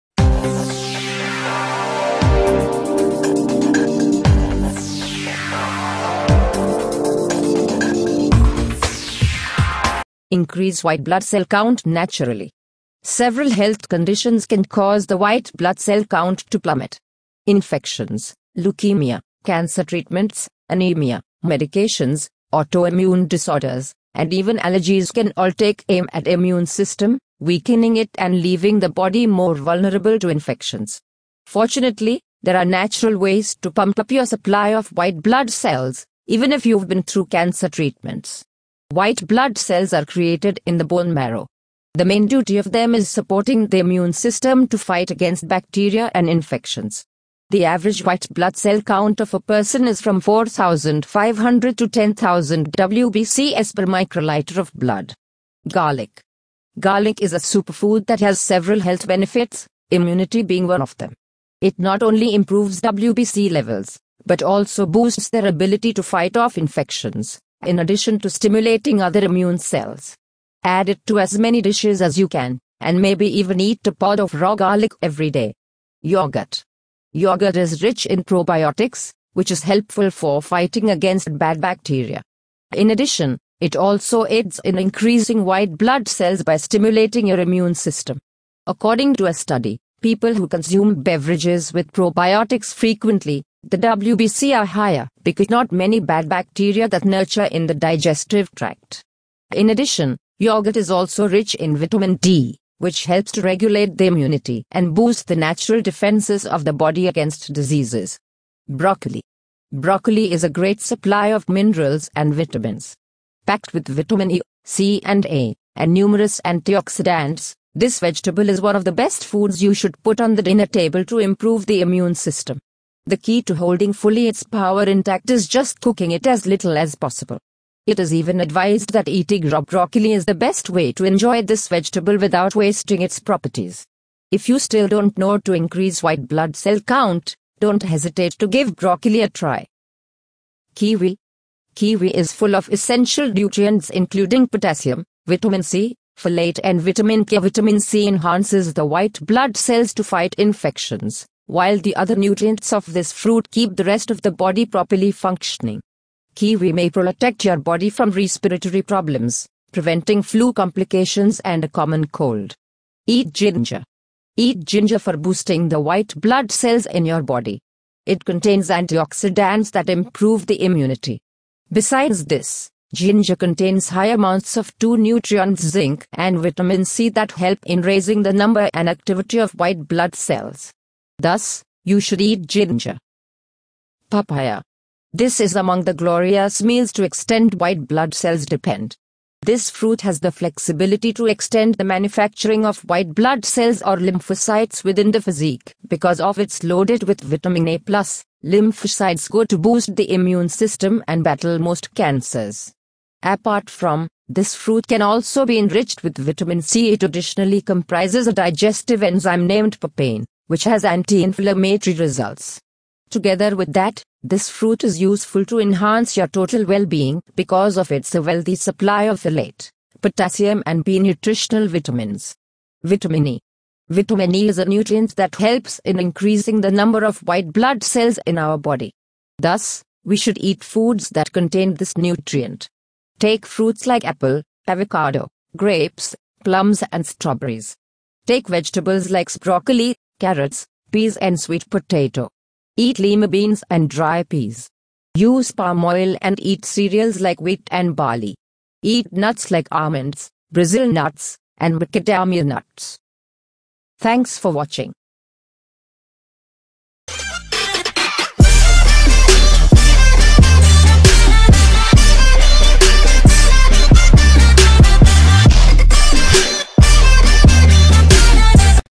Increase White Blood Cell Count Naturally
Several health conditions can cause the white blood cell count to plummet. Infections, leukemia, cancer treatments, anemia, medications, autoimmune disorders, and even allergies can all take aim at the immune system, weakening it and leaving the body more vulnerable to infections. Fortunately, there are natural ways to pump up your supply of white blood cells, even if you’ve been through cancer treatments. White blood cells are created in the bone marrow. The main duty of them is supporting the immune system to fight against bacteria and infections. The average white blood cell count of a person is from 4,500 to 10,000 WBCs per microliter of blood.